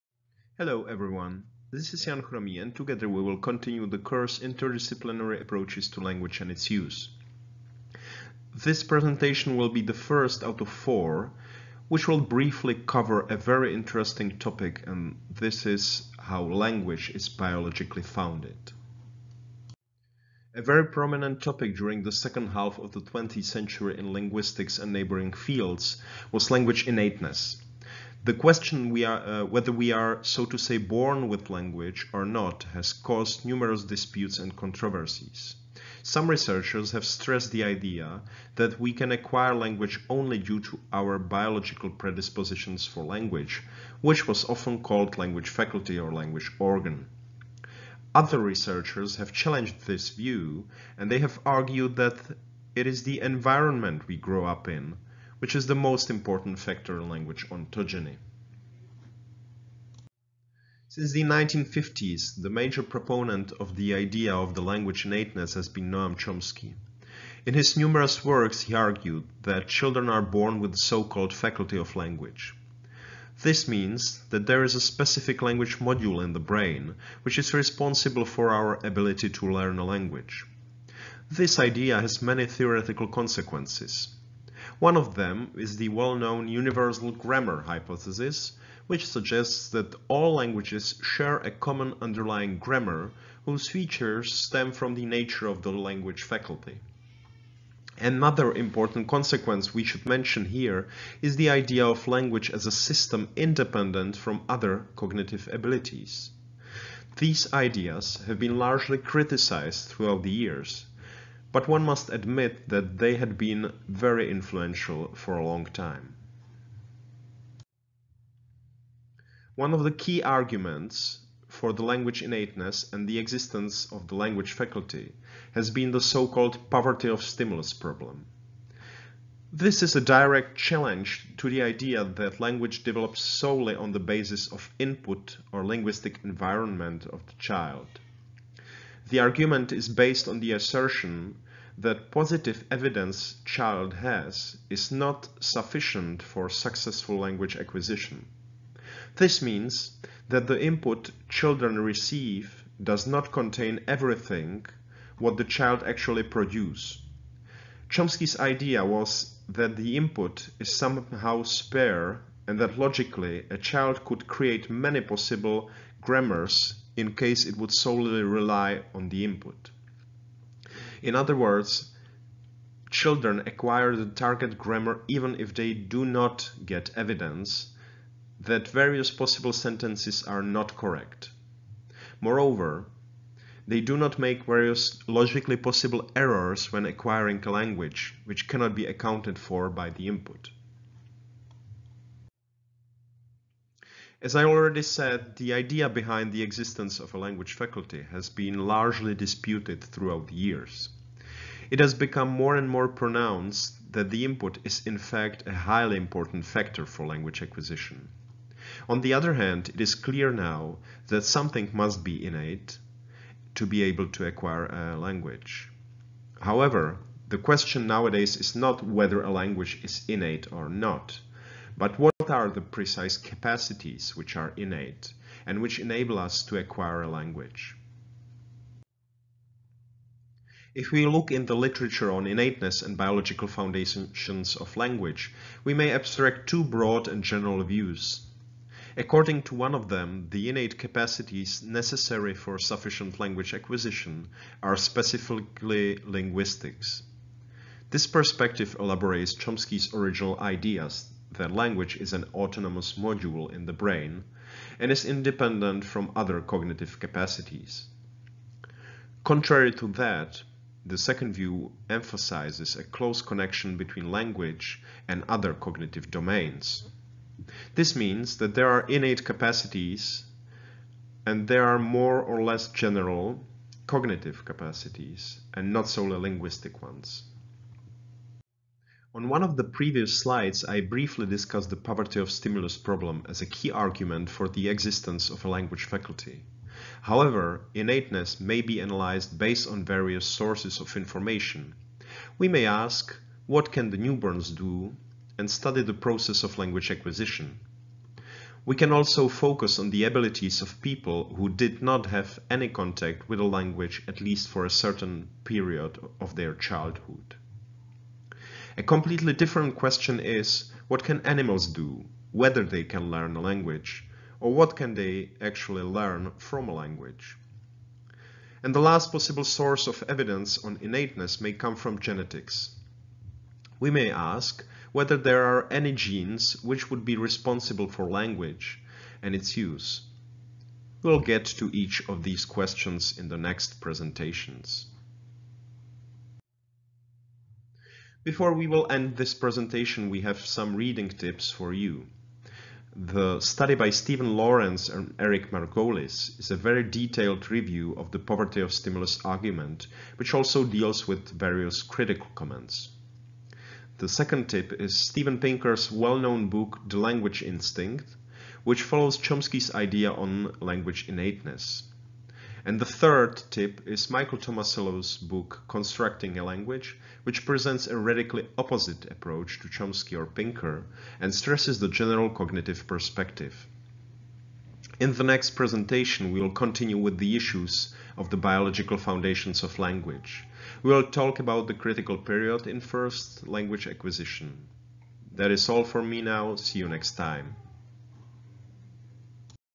01a Biological foundations of language
Interdisciplinary Approaches to Language and Its Use: Biological foundations of language

© Jan Chromý (2020)